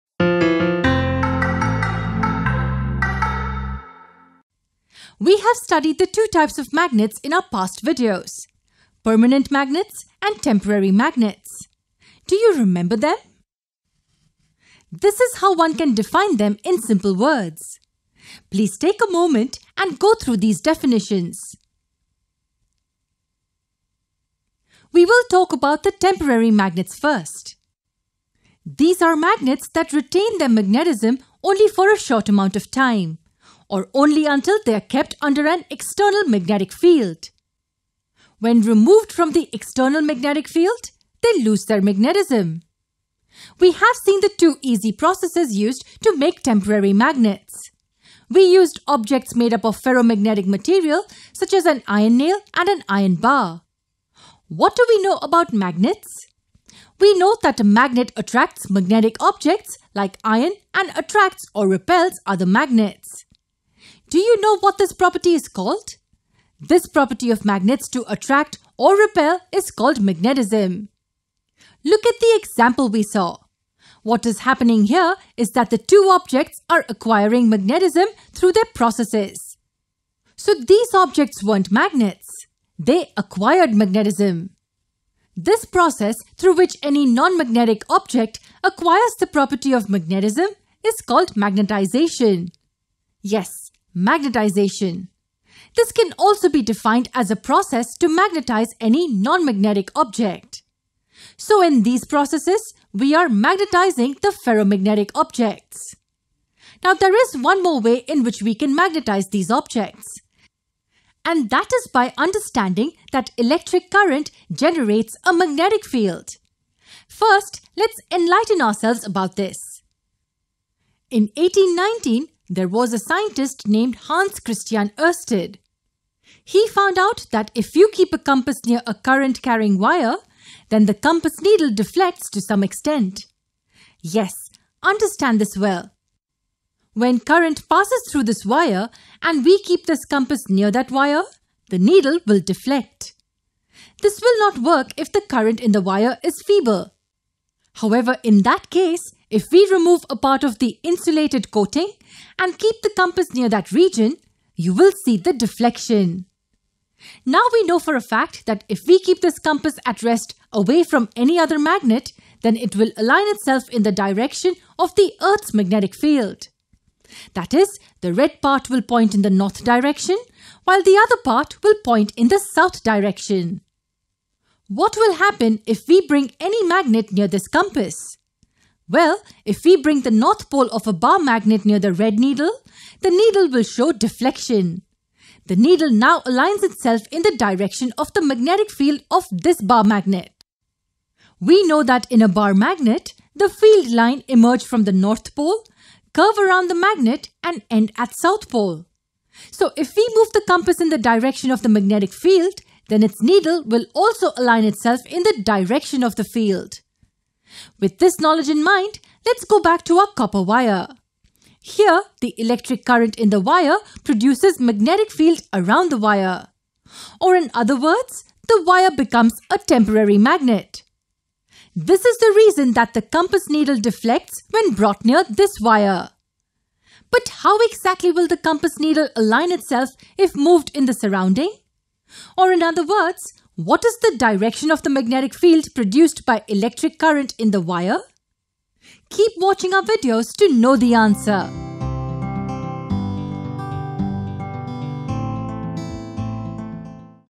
Magnetic Effects of Electric Current - Introduction | Don't Memorise
📅🆓NEET Rank & College Predictor 2024 We have learned quite a few processes to magnetise an Iron Nail in the topic Fun with Magnets. In this video, we will look at one more such process! Watch the video to know more.

To access all videos related to Electricity and Circuits, enroll in our full course now: